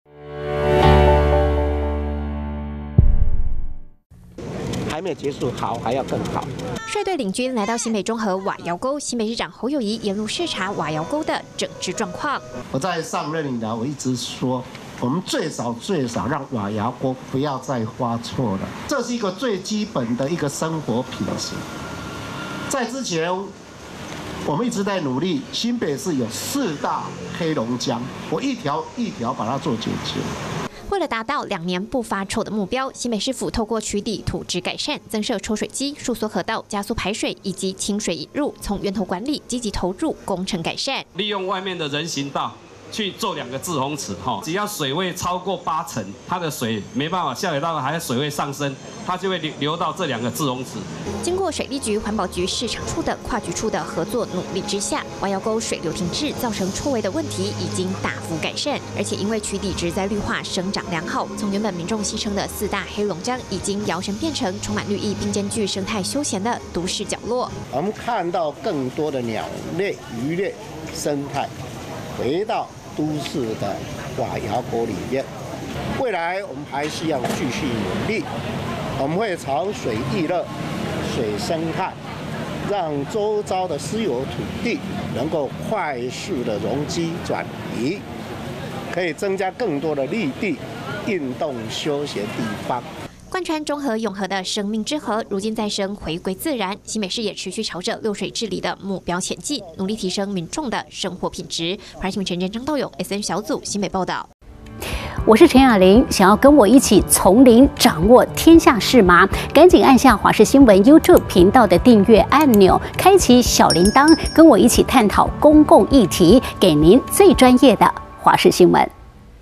視察瓦磘溝！ 侯：讓中永和生命之河回到最自然｜華視新聞 20230406
新北市中和區的瓦磘溝30多年來，惡臭影響周遭居民，新北市政府從源頭端做污水用戶接管、礫間處理及渠底改善，最後引入清水等整治措施，不到2年時間，瓦磘溝已經「蛻變再生」，新北市長侯友宜今(6)日也特別前往視察，他說看到更多的鳥類、魚類生態回到都市裡，未來會持續努力，續朝新北市「六水治理」的「水育樂」、「水生態」目標前進。